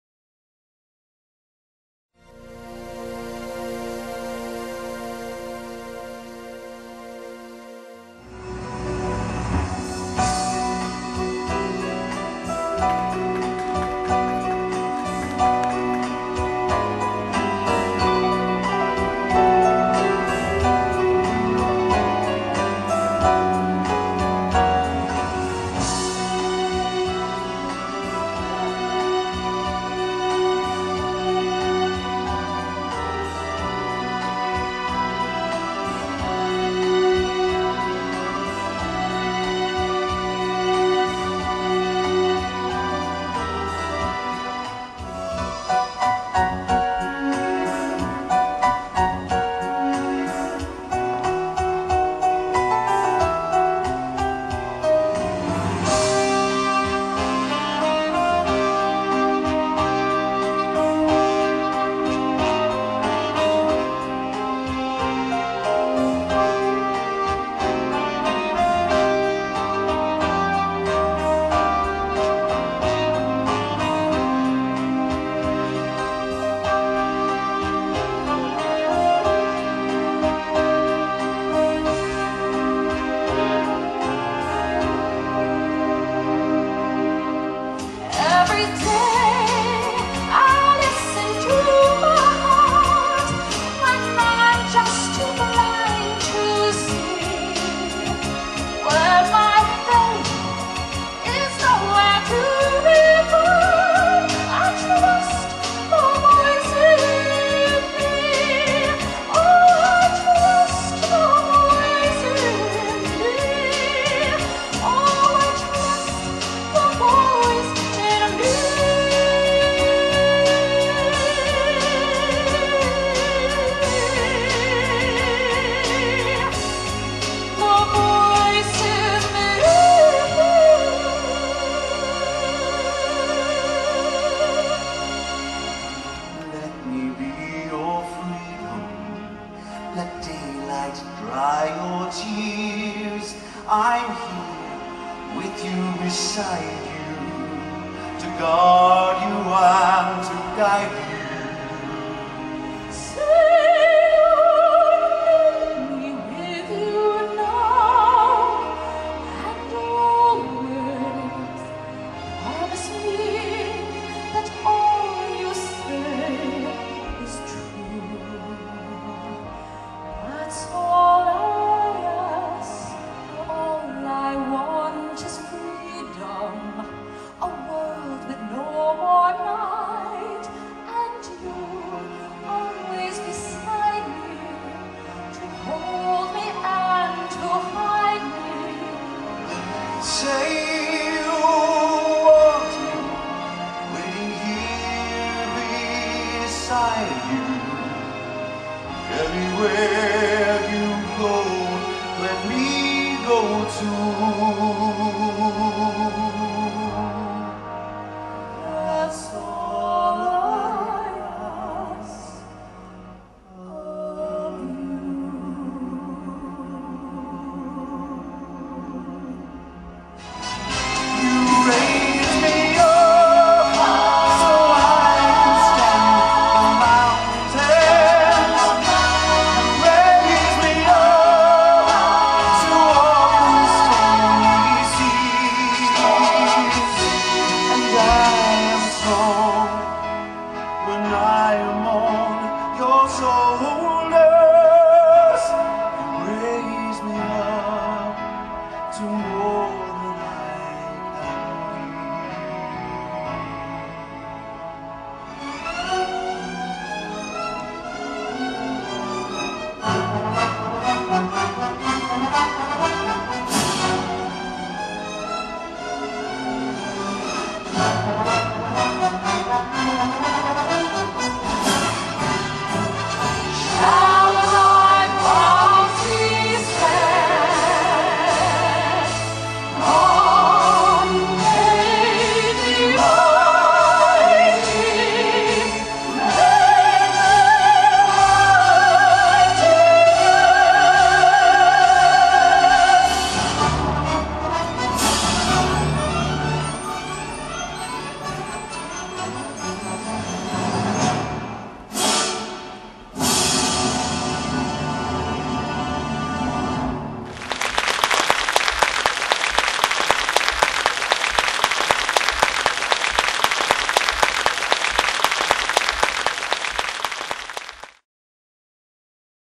Classical Moments
Production Show aboard 5 star cruise ship Asuka II.  Produced, Directed and choreographed by Gail Davies.